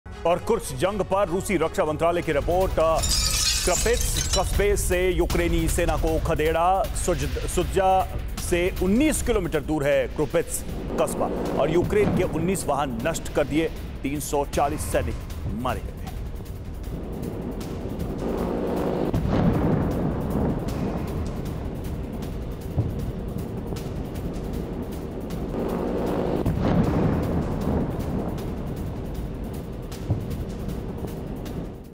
Russia Ukraine War: रूसी रक्षा मंत्रालय की रिपोर्ट...यूक्रेनी 340 सैनिकों की मौत | Putin | Biden
यूक्रेन के राष्ट्रपति जेलेंस्की की आक्रामक रणनीति रूस के लिए भारी पड़ रही है। कुर्स्क की जंग में यूक्रेन ने 8 दिन में ही उससे ज्यादा इलाका जीत लिया है जितना रूस की सेना ने बीते 250 दिन में जीता है। रूसी धरती पर कुर्स्क का मानचित्र तेजी से बदल रहा है, मील के पत्थर और झंडों का रंग भी साथ साथ बदलता जा रहा है। रूसी धरती पर यूक्रेन ने अबतक के सबसे बड़े हमले किए हैं। कुर्स्क के जीते हुए इलाकों की रक्षा करने के लिए यूक्रेन की लेडी ब्रिगेड तैनात हो चुकी है। नाटो और यूक्रेन के साझा ऑपरेशन के खिलाफ पुतिन ने एटमी नीति में परिवर्तन कर दिया है, देखिए कैसे पुतिन की ये रणनीति पूरे यूरोप की आहुति ले सकती है।